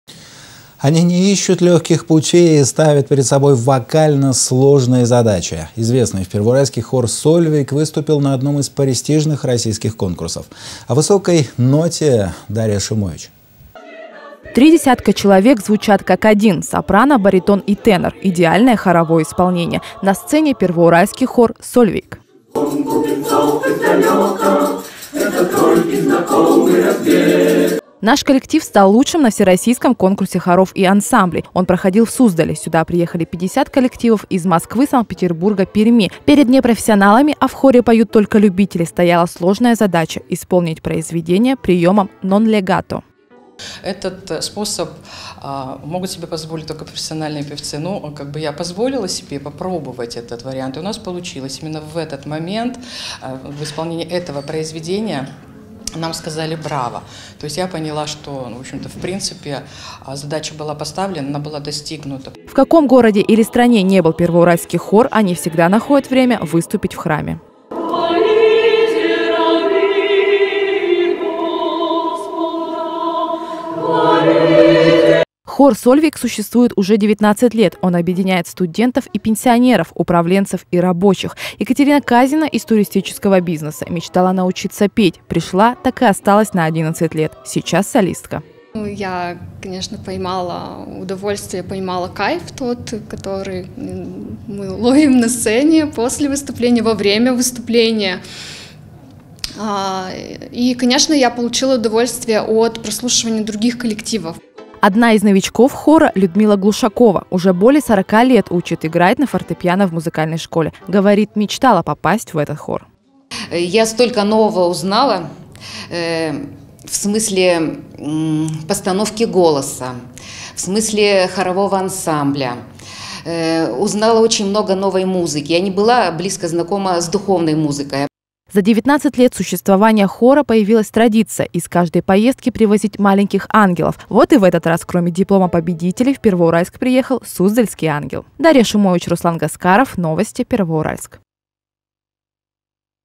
«Сольвейг» стал лучшим на всероссийском конкурсе
Они не ищут легких путей и ставят перед собой вокально сложные задачи. Известный в Первоуральске хор "Сольвейг" выступил на одном из престижных российских конкурсов. О высокой ноте - Дарья Шумович.
Три десятка человек звучат как один - сопрано, баритон и тенор - идеальное хоровое исполнение. На сцене первоуральский хор "Сольвейг".
Наш коллектив стал лучшим на всероссийском конкурсе хоров и ансамблей. Он проходит в Суздале. Сюда приехали пятьдесят коллективов из Москвы, Сант-Петербурга, Перми. Перед не профессионалами - а в хоре поют только любители - стояла сложная задача - исполнить произведение приемом "нон легато".
Елена  ШЕСТАКОВА, руководитель хора "Сольвейг": «Этот способ  могут себе позволить только профессиональные  певцы, но я позволила себе попробовать этот вариант и у нас получилось. Именно в этот момент в исполнении этого  произведения нам сказали "Браво, задача была поставлена,  она  была  достигнута».
В каком бы городе или стране не был первоуральский хор, они всегда находят время выступить в храме.
Хор "Сольйвег" существует уже 19 лет. Он объединят студентов и пенсионеров, управленцев и рабочих. Екатерина Казина из туристического бизнеса. Мечтала научиться петь. Пришла, так и осталась на 11 лет. Сейчас солистка.
Екатерина КАЗИНА, участница хора: «Я конечно поймала удовольствие, поймала  кайф, который мы ловим на сцене, после  выступления, во время выступления. И конечно я получила удовольствие от прослушивания других коллективов».
Одна из "новичков" хора - Людмила Глушакова. Уже более 40 лет учит играть на фортепиано в музыкальной школе. Говорит, мечтала в этот хор попасть.
Людмила  ГЛУШКОВА, участница хора: «Я столько нового узнала, в смысле постановке  голоса, в смысле хорового ансамбля,  узнала  очень  много  новой  музыки. Я  не была близко знакома с духовной музыкой».
За 19 лет существования хора появились традиция, из каждой поездки привозить маленьких ангелов. Вот и в этот раз, кроме диплома победителей, в Первоуральск приехал суздальский ангел.

Дарья Шумович